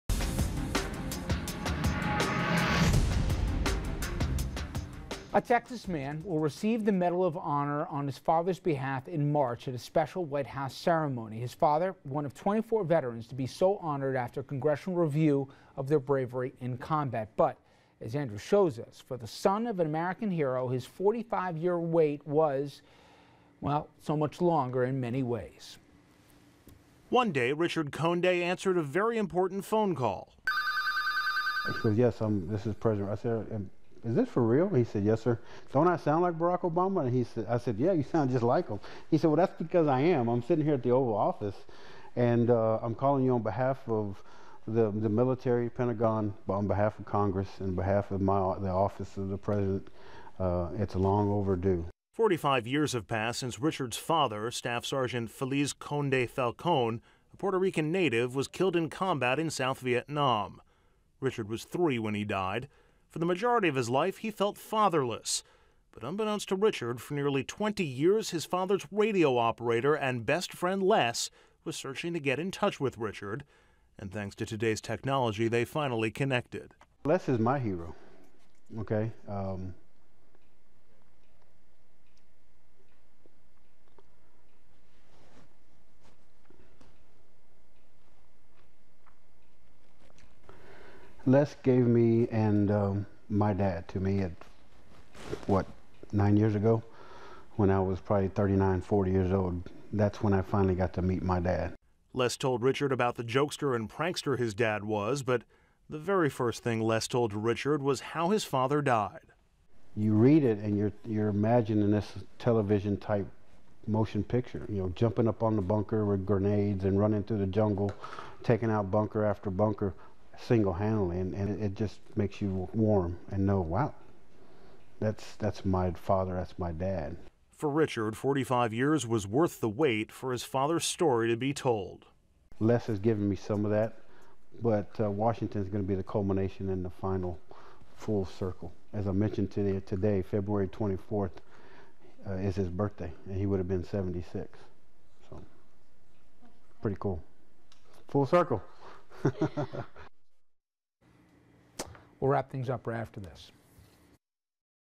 A Medal of Honor - And So Much More
RFL has the special story of Richard Conde, who will receive the Medal of Honor 45 years after his father was killed in combat in South Vietnam.